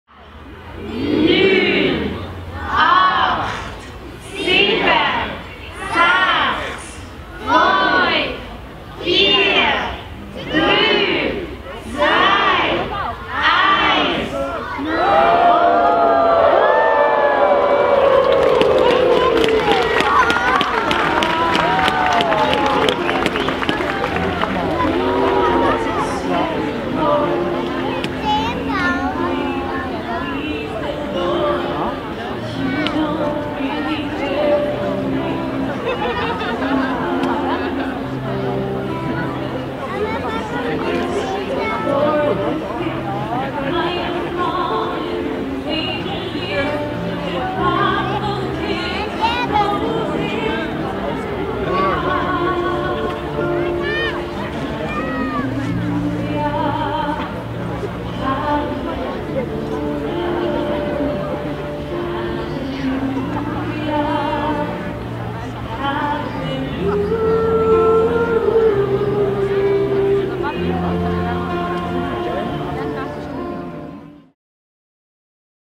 Im Baur au Lac wird Zürichs höchster Weihnachtsbaum eingeweiht
Feierliche Eröffnung der Vorweihnachtszeit: Christmas Tree Lighting im Park des Baur au Lac.